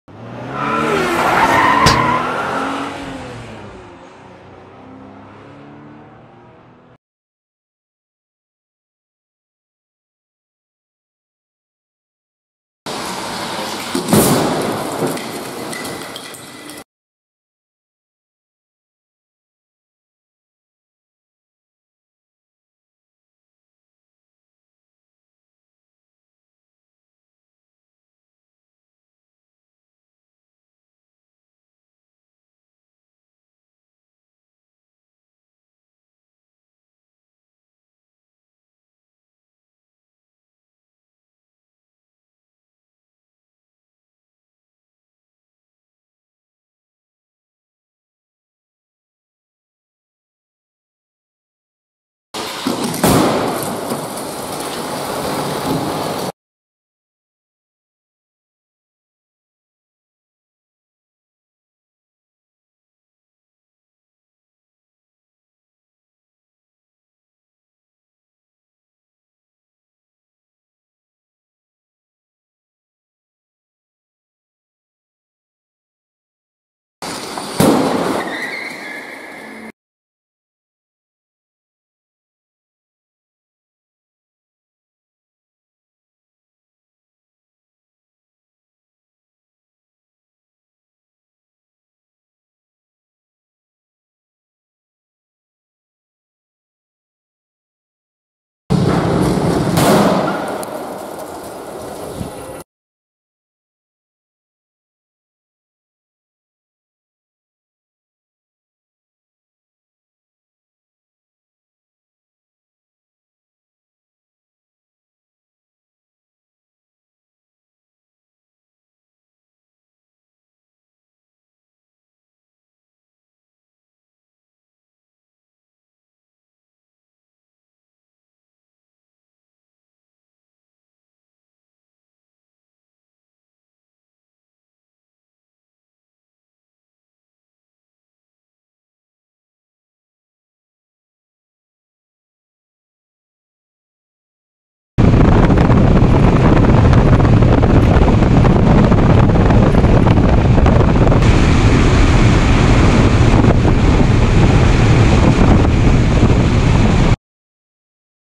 Volkswagen up! - Seat Mii - Skoda Citigo - 2019 - Crash test Euro NCAP
Il Crash Test Euro NCAP e la verifica dei sistemi di sicurezza attiva avanzata su Volkswagen up!, Seat Mii e Skoda Citigo